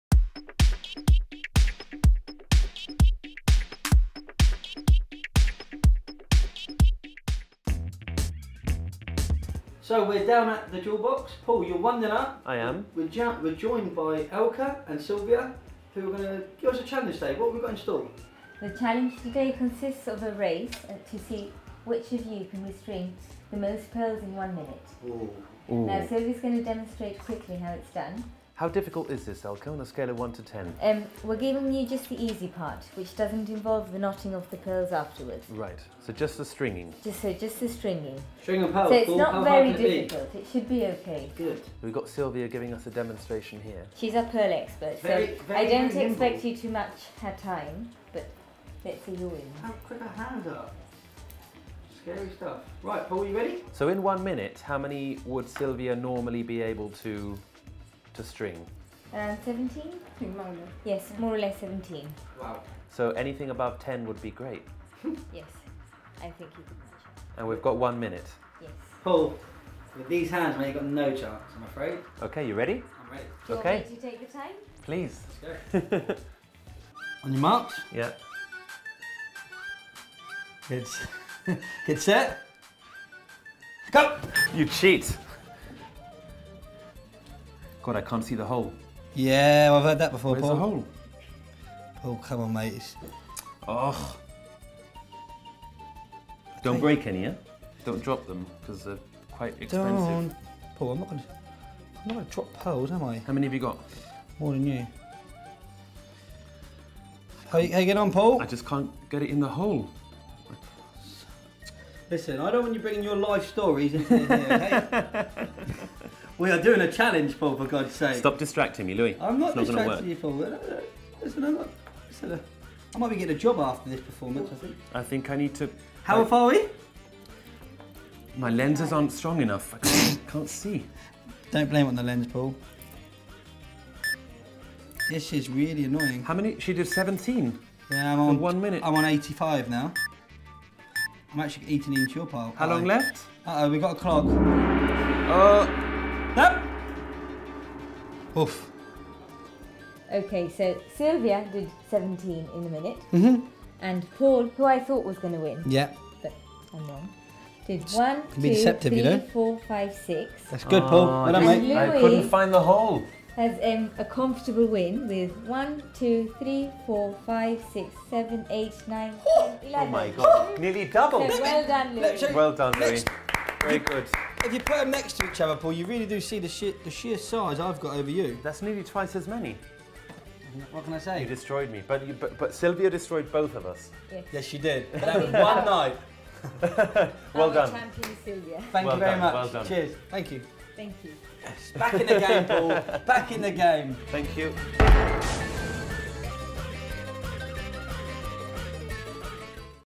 Paul v Louis Challenge (Second Round)
Paul and Louis continue their Open Day Special Challenge to raise funds for the GBC Open Day, thanks to Elka and the team at The Jewel Box